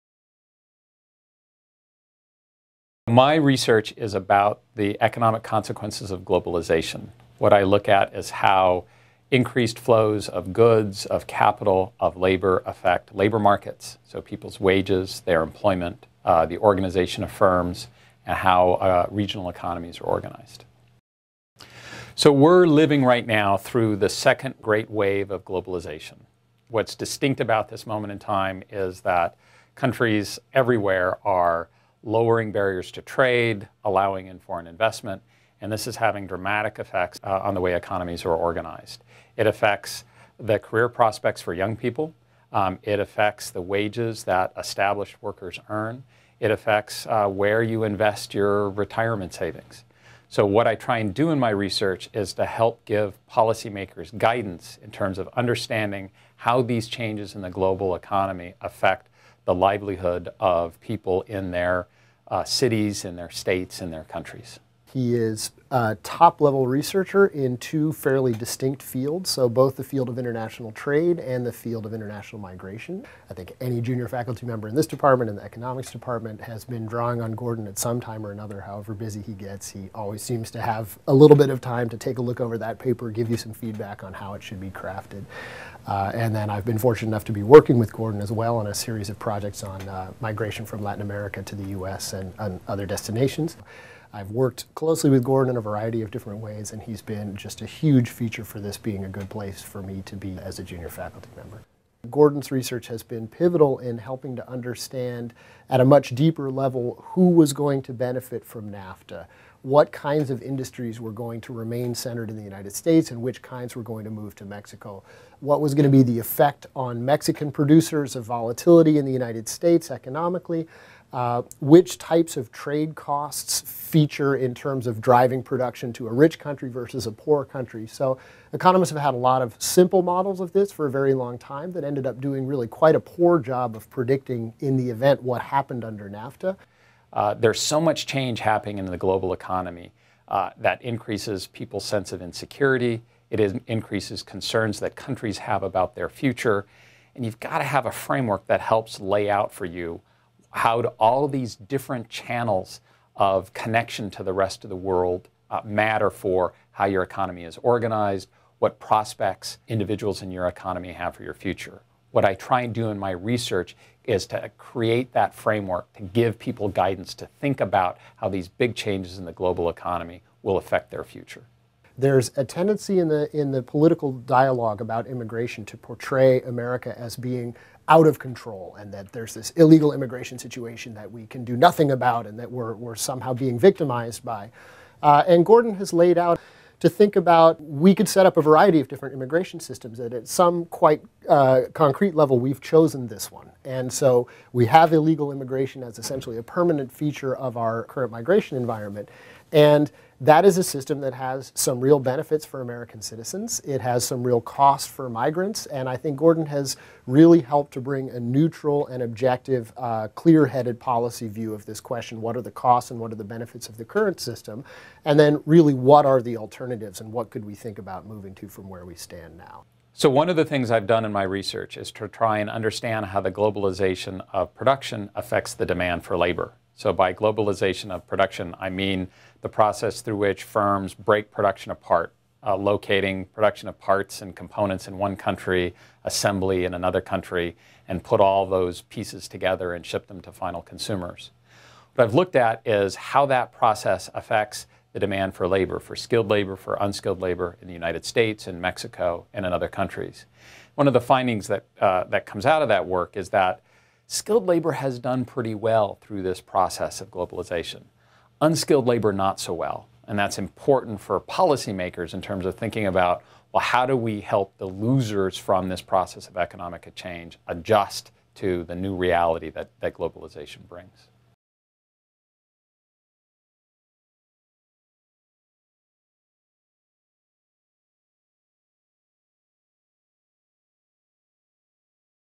Gordon Hanson - 2011 Chancellor's Associates Faculty Excellence Award
Gordon Hanson, Professor of International Economics
Excellence in Research in Humanities and Social Sciences
One of the most productive and highly cited economists in the United States, Hanson is considered one of a handful of superstars in the field of international economics. A pioneer in developing "the new empirical economics," he is an authority on international trade, international migration, economic development and foreign investment, applying economic analysis to understanding the social implications of timely issues. Hanson is committed to the dissemination of his findings to policy makers and the global community.

"Professor Hanson is one of his generation's pioneers who have combined very original ways of investigating problems empirically with astute innovations in theorizing," said Peter Cowhey, dean of the School of International Relations and Pacific Studies. "His recent work will stand among the most important contributions to the field of international economics and migration."